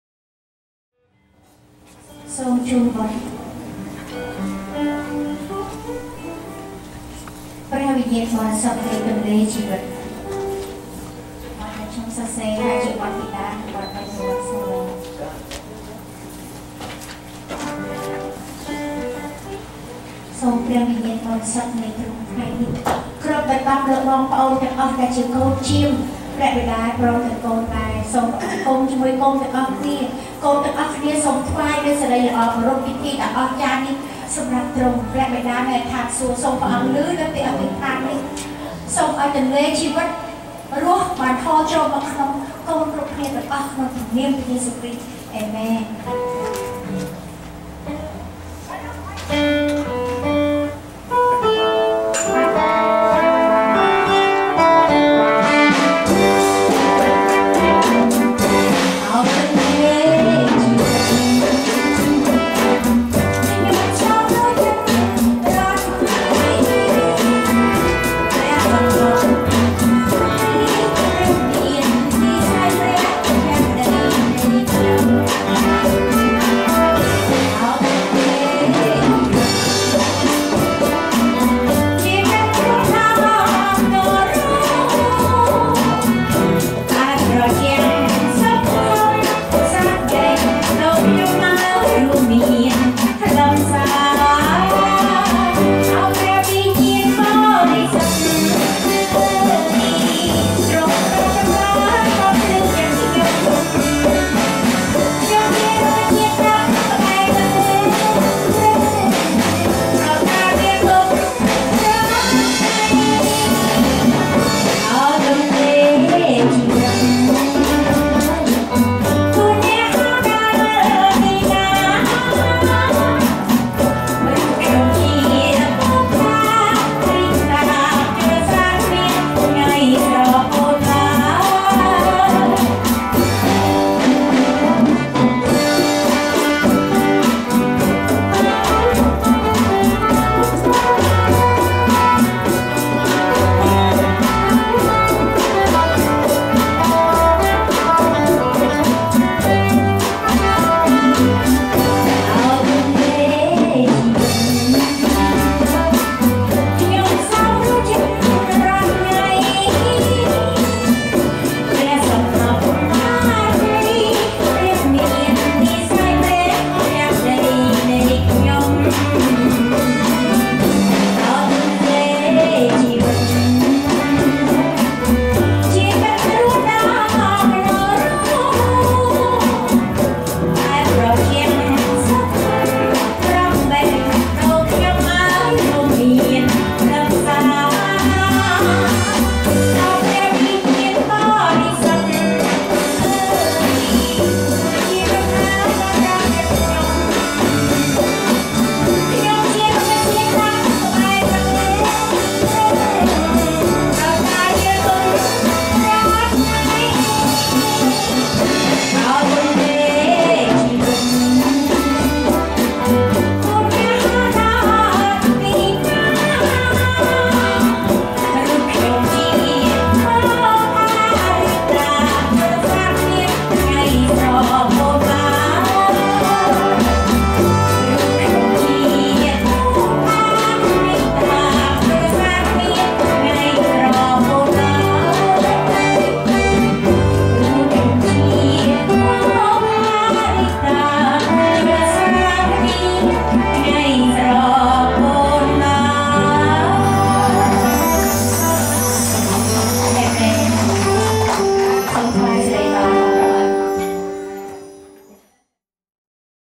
Khmer Christian Song by Ms. Seng Botum at Cambodian Harvest Church
CAMBODIAN HARVEST CHURCH
SIGNAL HILL CALIFORNIA USA

PENTECOST CELEBRATION
Special Song by Ms. Seng Botum